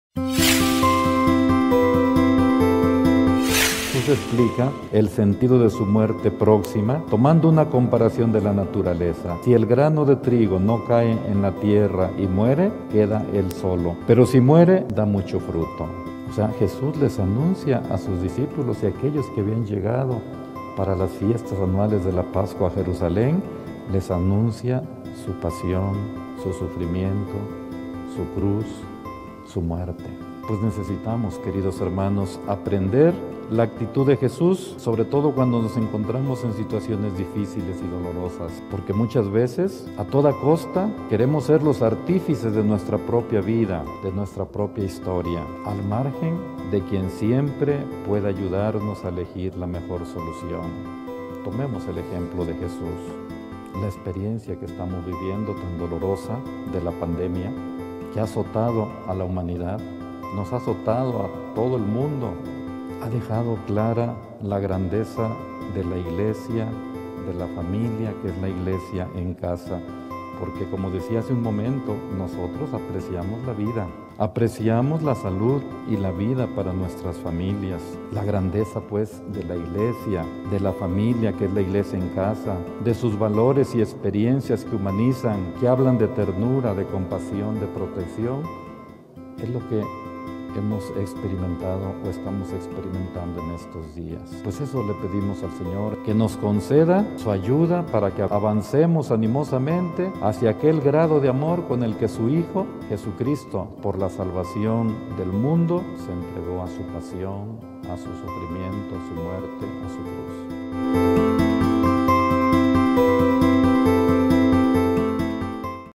Reflexión Mons. Víctor Sánchez Espinosa - 21 de marzo 2021 - Arquidiócesis de Puebla
Reflexión Mons. Víctor Sánchez Espinosa en el V Domingo de Cuaresma - 21 de marzo 2021 - Arquidiócesis de Puebla

Descarga la app Arqui Puebla:
- Android: